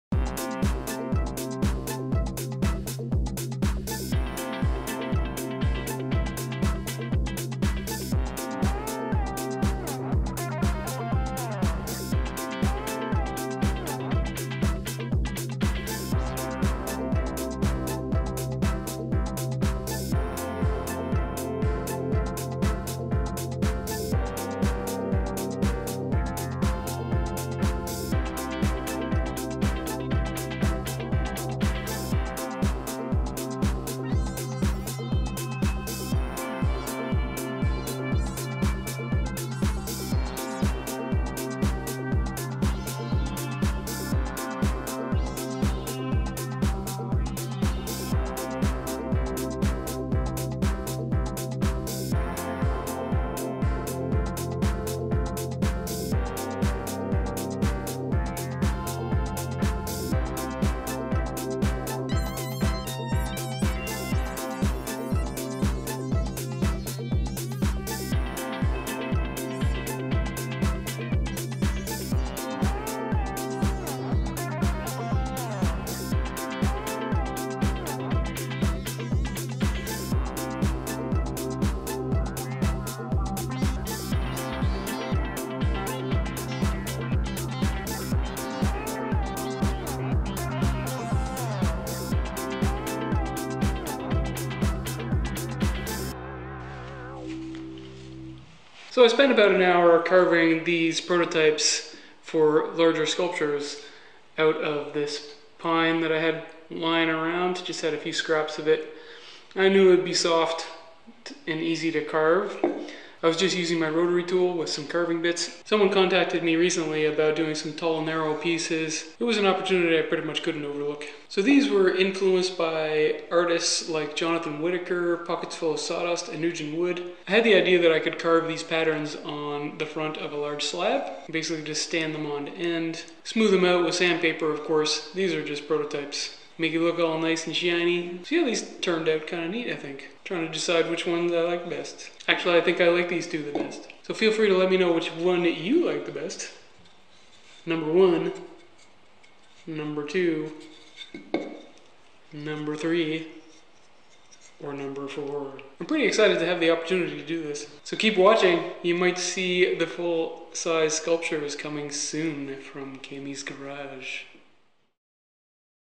Power Carved Sculpture Prototypes - Woodworking, Art, Sculpture
Recently, someone contacted me about doing two large art pieces. It is an opportunity I could not overlook. These are essentially sketches or prototypes of larger sculptures to show to the patron.